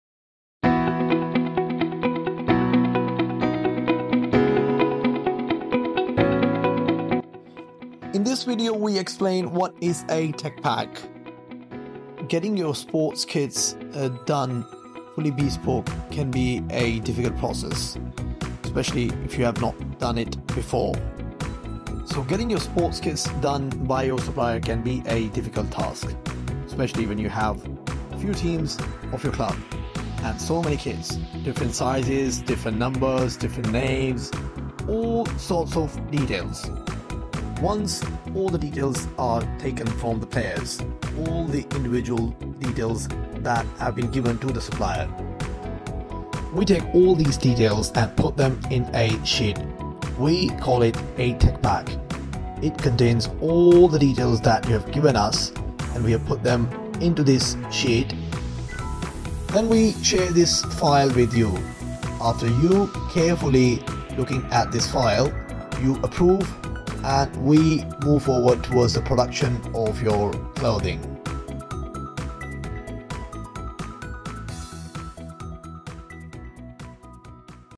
What is a Tech-pack ? how bespoke clothing is made ? How are sports kits made ?
It's a video which explains about the tech-pack. A tech-pack is a technical file which has all the details of a bespoke clothing order. In such an order there can be size details, total number of items and what kind of customisation is to be done on the clothing ? Embroidery, printing and dye-sublimation details explained in this sheet.

    This contains all the data from a customer/club/business that want it's clothing done with particular details.

GI-UK
premium sports clothing supplier